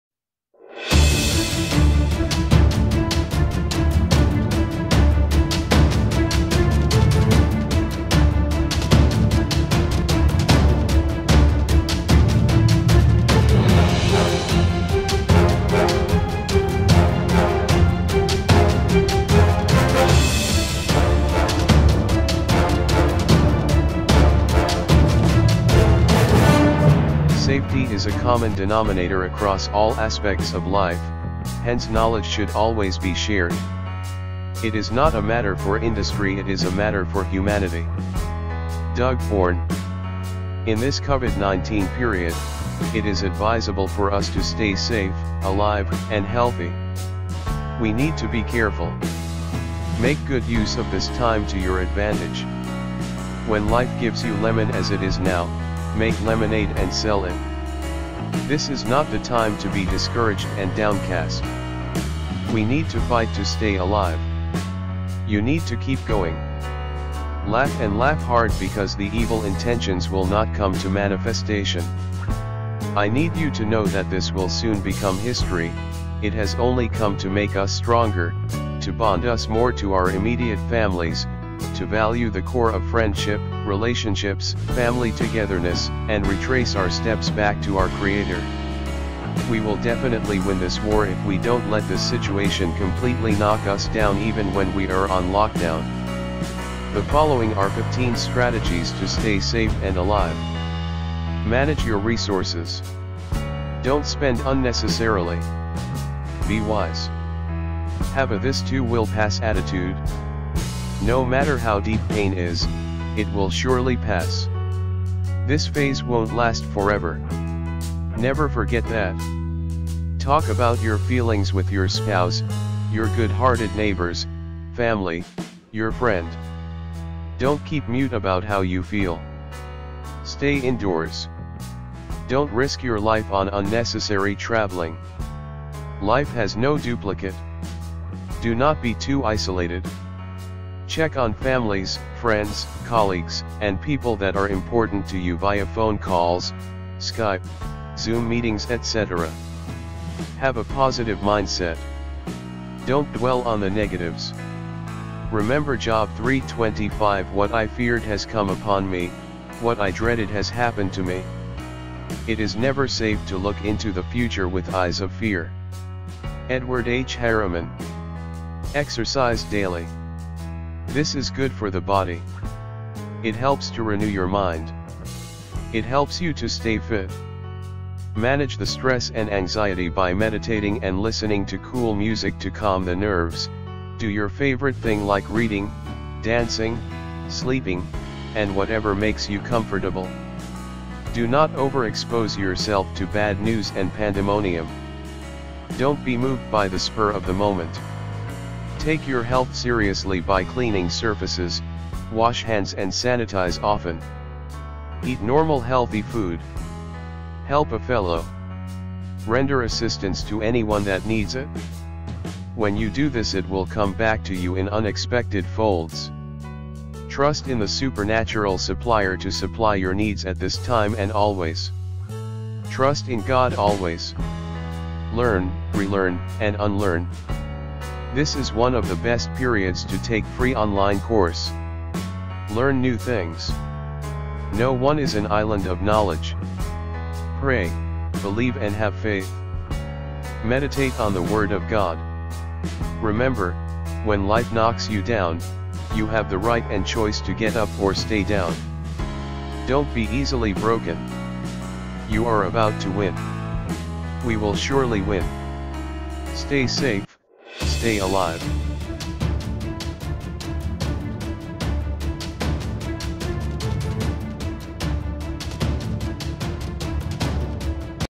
COVID-19: STAY SAFE, STAY ALIVE
Hi viewers,
We are in a period where everyone is on lockdown. In this period, the major focus is on how to stay safe and alive. The video focuses on 15 strategies to stay safe and alive.

Kindly watch, comment, share and click on the subscription button.